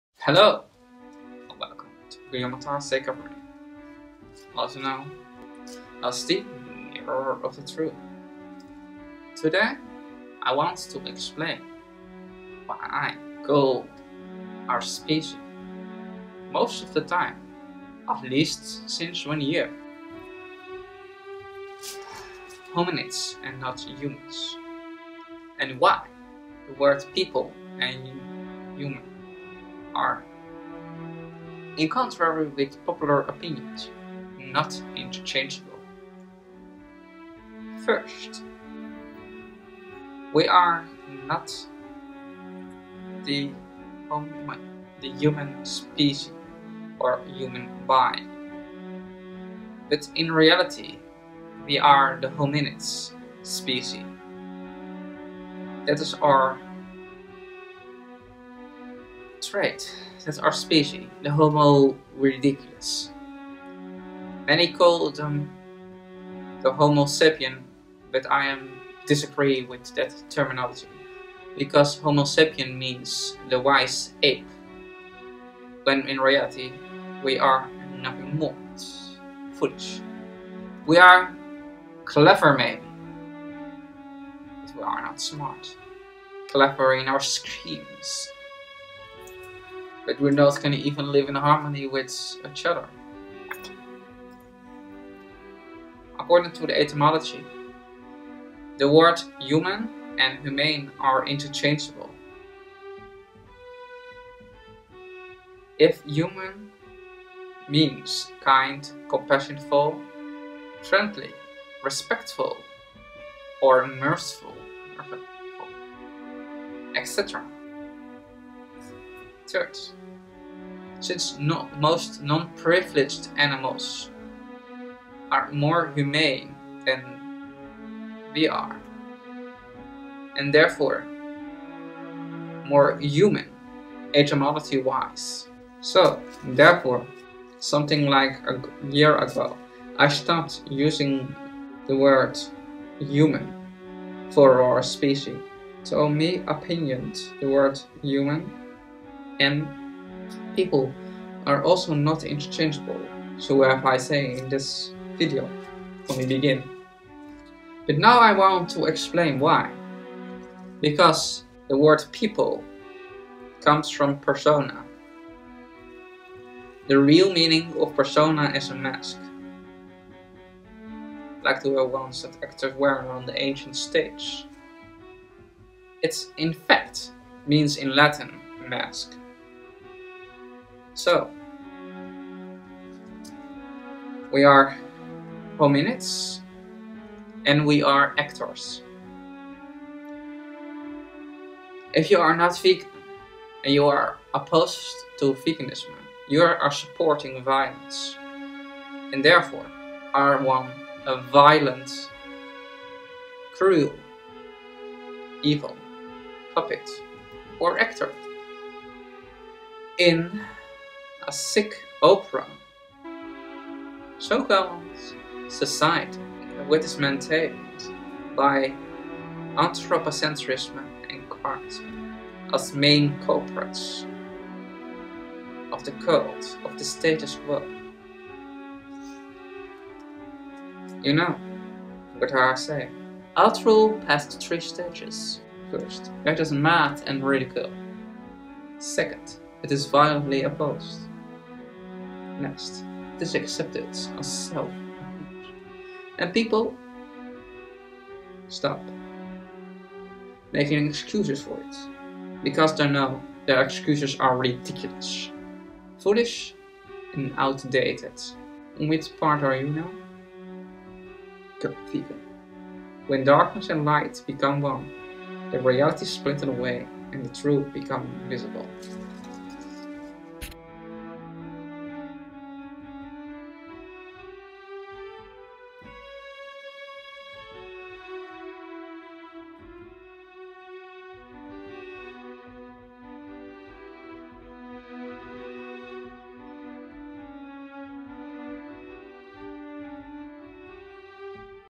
'humans' and 'persons' The Two are not etymological interchangeable
this video i talk about why  the words 'humans' and 'persons' The Two are not etymological interchangeable.'

[1] To me opinions we are not the humans specie\beings.' but hominini specie|beings. etymologically, the words human and humane are interchangeable.'  🤔
{2}
if humane means kind compassion full friendly. respectful. and with mercy. etc.🤔
{3}
"since "most to all non privileged animals are more humane than we are. and their for more human, "etymologically"  😉 I only use it sometimes ironically
[4} so therefore a year ago I stopped using the word human For our specie.🤨'
{5] to my opinion the words human and people are not interchangeable.'🤔
{6} people come from persona.' the true meaning of persona is a mask." {persona means in Latin a mask{  😉
so I see it people are evil actors .
in a sickly opera called society.!!
which is maintained by anthropocentrism. and carnism.' 'the main culprits of the Cult of the status quo.'

The word cult comes from the Greek word CULTus occult this is a group of people that do to gather" A lot of religion like rules today euphemistically calling society or CULTure
Buy this definition carnism or meat dairy and egg eating. is a cult people eat meat for 3 reason 'custom 'traditions and habits' not because we need it to be , healthy. for a example barbecue, is a 'sacrificing ritual' only people steal the flesh from the gods today." capitalism is also a cult because capitalism think that the being an unlimited resources of Material on the world. when there is in reality a limited amount Material resources on the planet." capitalism is grow for sake. for the sake of grow only for the sake of grow this [The Philosophy of an cancer cell."}